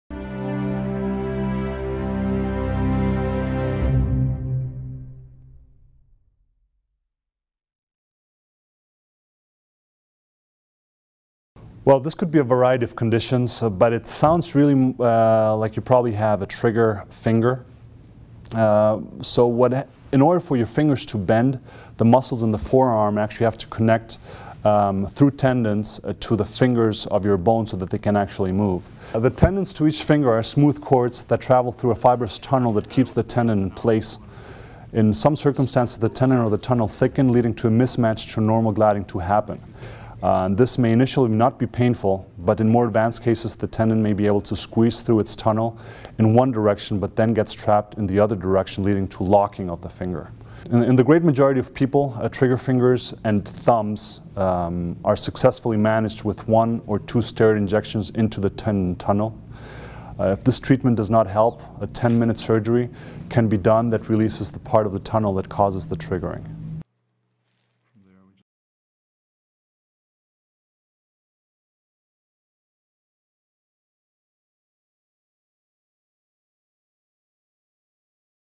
ASK UNMC! I noticed one of my fingers locks after making a fist, what is wrong?
Philipp Streubel, M.D., UNMC College of Medicne